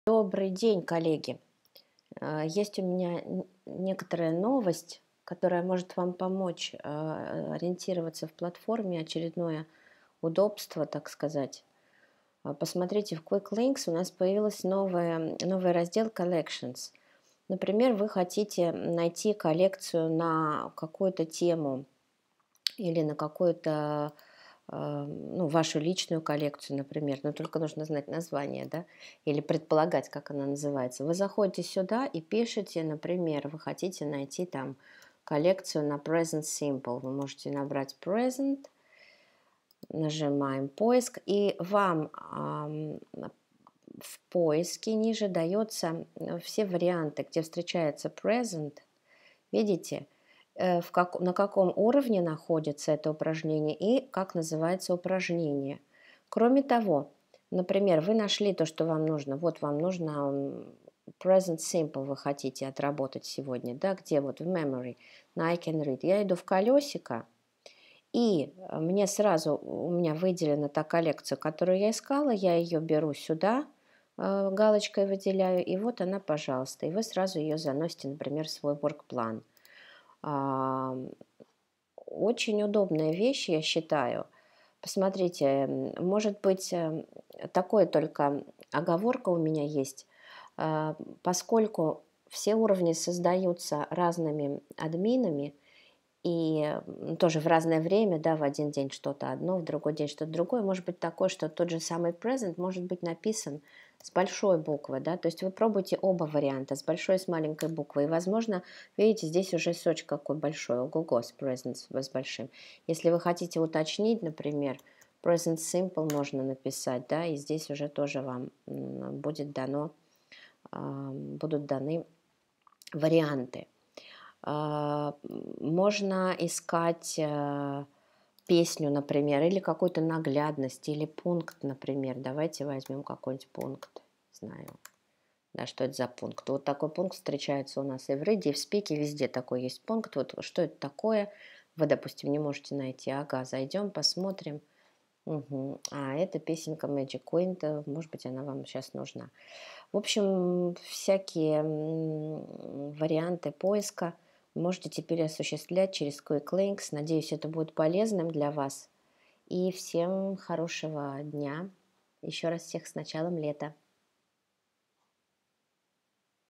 Search for Collections on PIKIMONI
В этом видео о том, как найти нужную коллекцию, пункт из Книги для Учителя, песню! в каком упражнении можно отработать ту или иную грамматическую конструкцию - с этим вопросом поможет поиск Collections из раздела Quick Links.